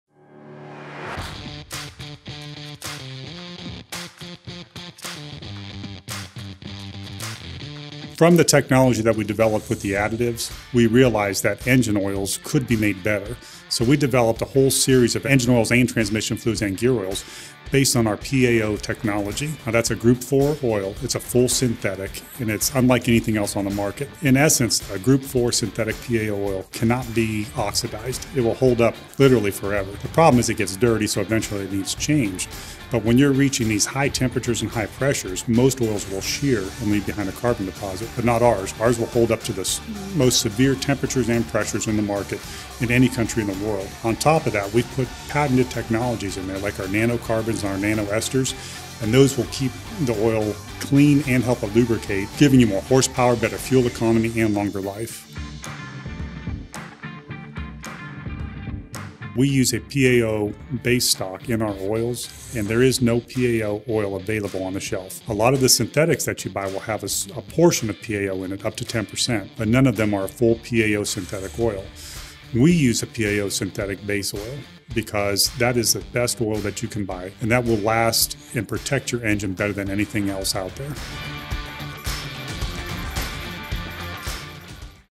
Hot Shot's Secret made a better engine oil
Learn about what makes Hot Shot's Secret PAO engine oils better than anything else available on the shelf today.